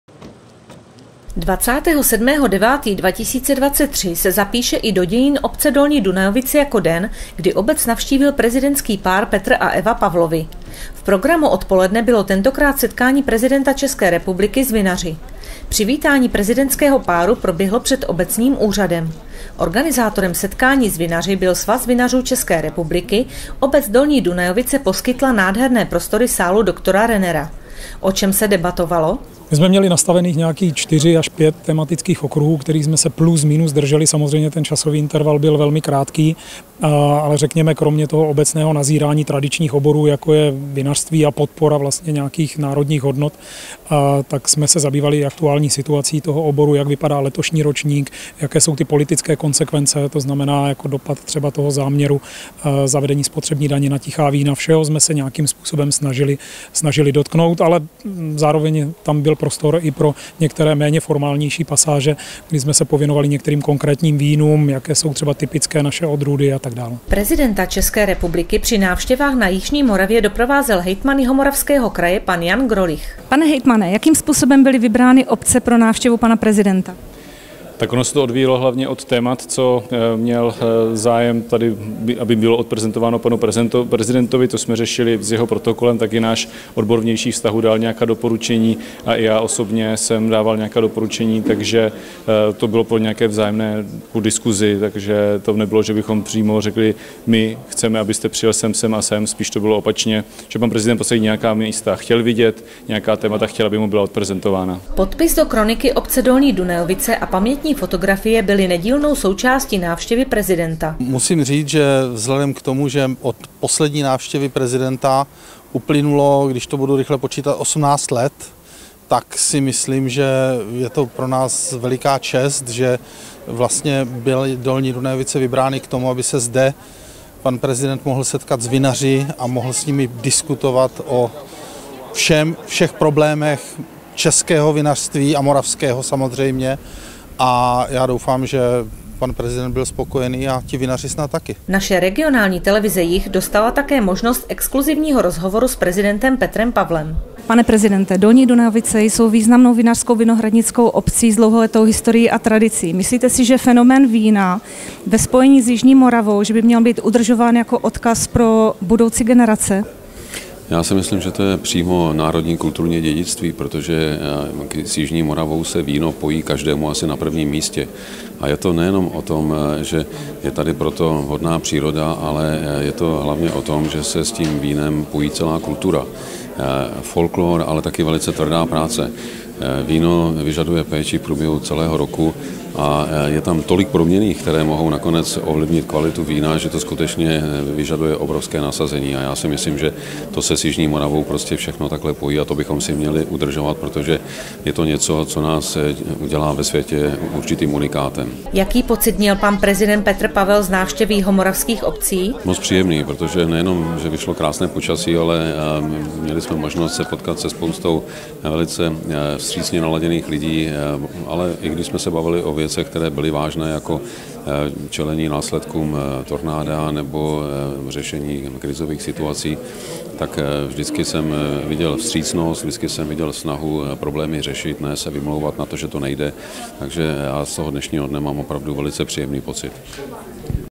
V Dolních Dunajovicích prezident České republiky Petr Pavel debatoval s vinaři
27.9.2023 se zapíše i do dějin obce Dolní Dunajovice jako den, kdy obec navštívil prezidentský pár Petr a Eva Pavlovi. V programu odpoledne bylo tentokrát setkání prezidenta České republiky s vinaři. Přivítání prezidentského páru proběhlo před obecním úřadem. Organizátorem setkání s vinaři byl Svaz vinařů ČR, obec Dolní Dunajovice poskytla nádherné prostory sálu Dr. Rennera. O čem se debatovalo?